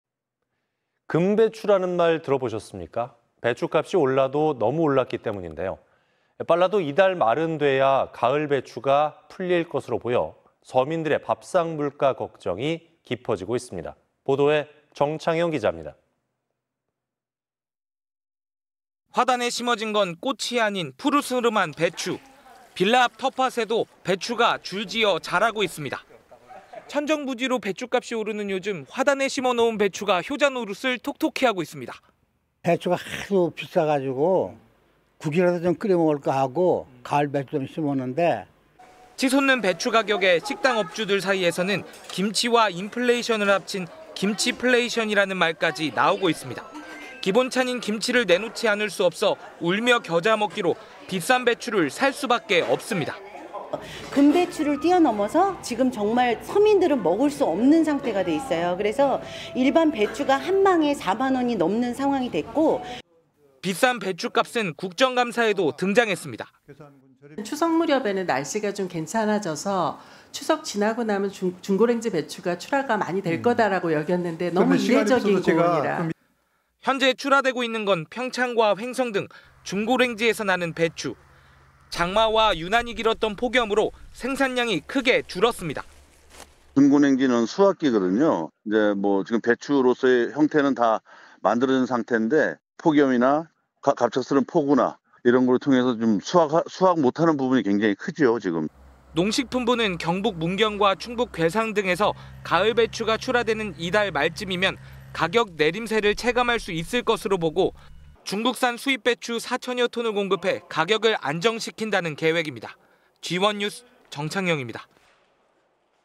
[G1뉴스]손 떨리는 배춧값..금배추 언제까지?
'금배추'라는 말 들어보셨습니까.

 배춧값이 올라도 너무 올랐기 때문인데요.

 빨라도 이달 말은 돼야 가을배추가 풀릴 것으로 보여 서민들의 밥상 물가 걱정이 깊어지고 있습니다.